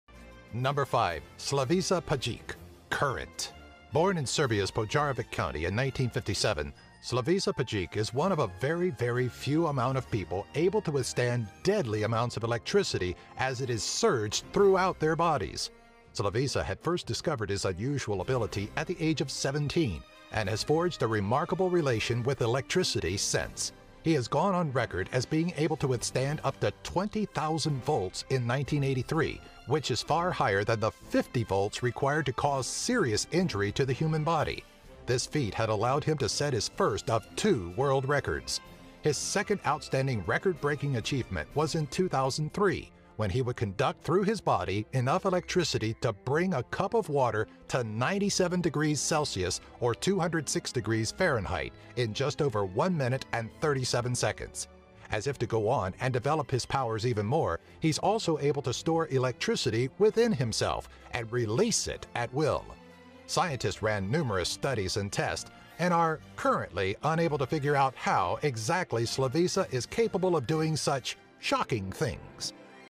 People With Real Super power. Slavisa pajkic-"current".#5. .
Top 10 Amazing People With Real Super Powers! We've all had those dreams, the thought of what it would be like to have super powers. Well, for these select individuals around the world, it's more than just a dream. From super strength to inhuman perception, we're counting down our picks for the Top 10 Humans With Real Super Powers!

10. Stephen Wiltshire - "Shutter"
9. Miroslaw Magola - "Magnetix"
8. Dean Karnazes - "Marathon Man"
7. Liam Hoekstra - "Herculad"
6. Daniel Tammet - "Brain Wave"
5. Slavisa Pajkic - "Current"
4. Tori Allen - "Monkey Girl"
3. Isao Machii - "Ronan"
2. Scott Flansburg's - "Calcula-TOR!"
1. Stig Severinsen - "Gill"